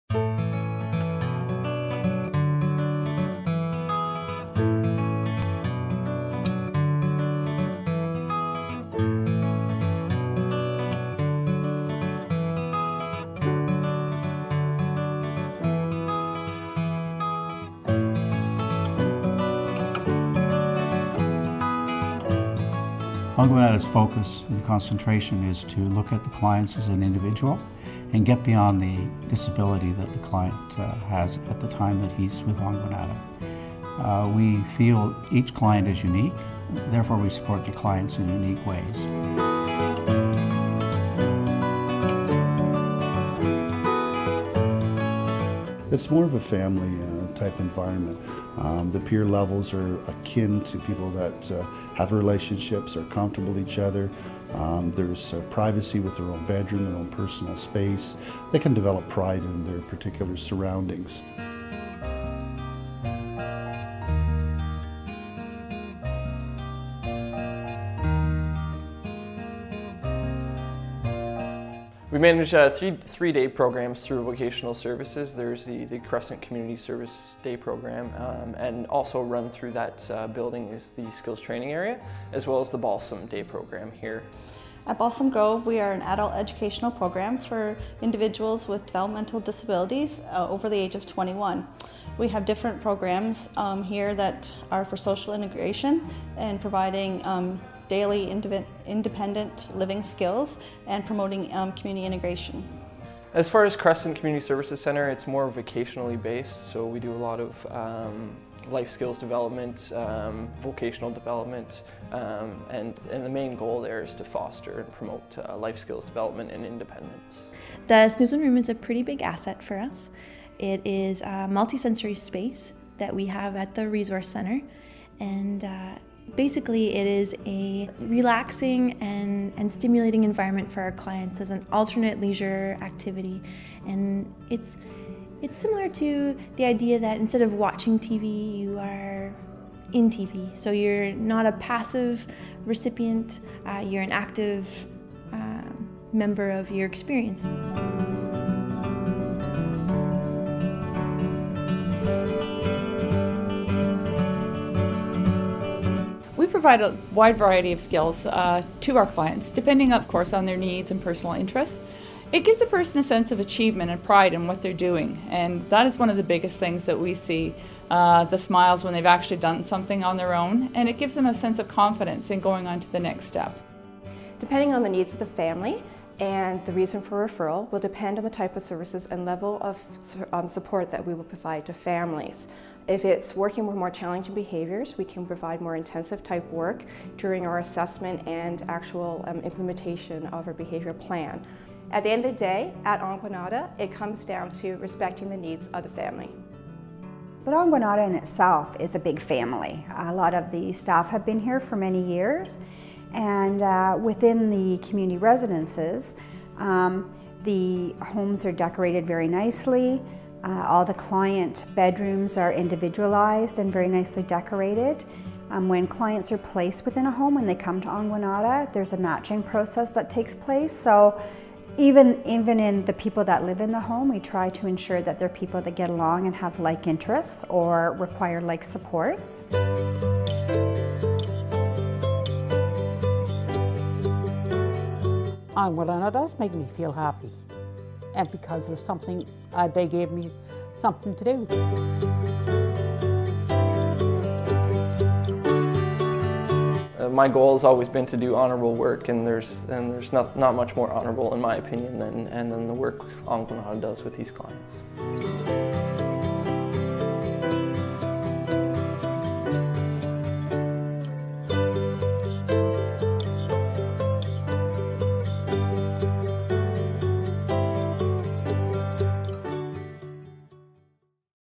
Ongwanada
We produced a video that highlights the programs and services offered by Ongwanada, an organization that supports people with developmental disabilities in Kingston, Ontario and the surrounding area.  We originally created a 15-minute video which Ongwanada uses for events and presentations but they also wanted a shorter version for the web. This is the shorter version we created for them giving them two videos from one project for multiple uses.